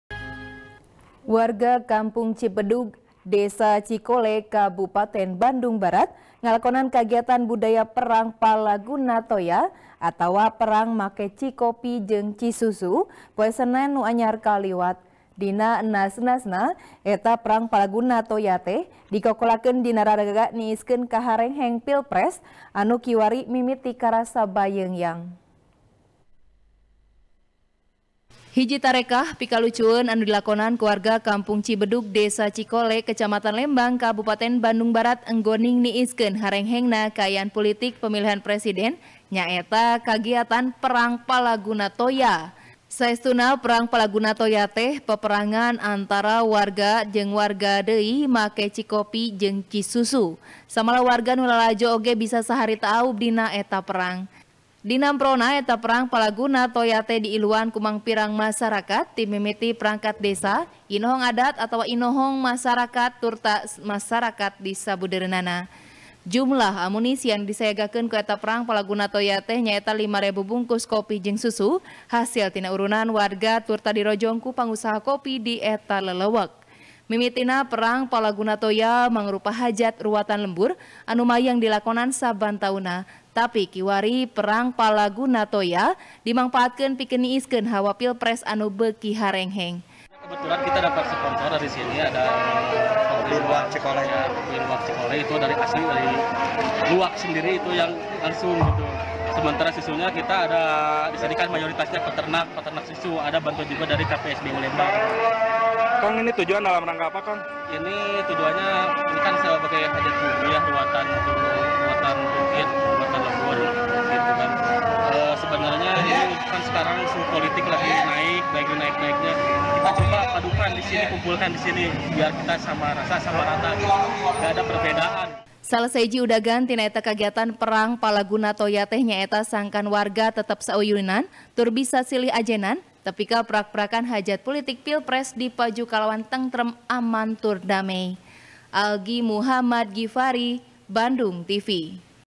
Niiskeun Kaharengheng Pilpres, Warga Cikole Nyorang Perang Palaguna Toya | TP | BANDUNGTV
TP | BANDUNGTV | ELBY NOPITRI | Warga Kampung Cibedug, Desa Cikole, Kabupaten Bandung Barat, Ngalakonan Kagiatan Budaya Perang Palaguna Toya, Atawa Perang Make Cikopi Jeung Cisusu, Poe Senen Nu Anyar Kaliwat. Dina Enas-Enasna, Eta Perang Palaguna Toya Teh, Dilakonan Dina Raraga Niiskeun Kaharengheng Pilpres Anu Kiwari Mimiti Karasa Bayeungyang.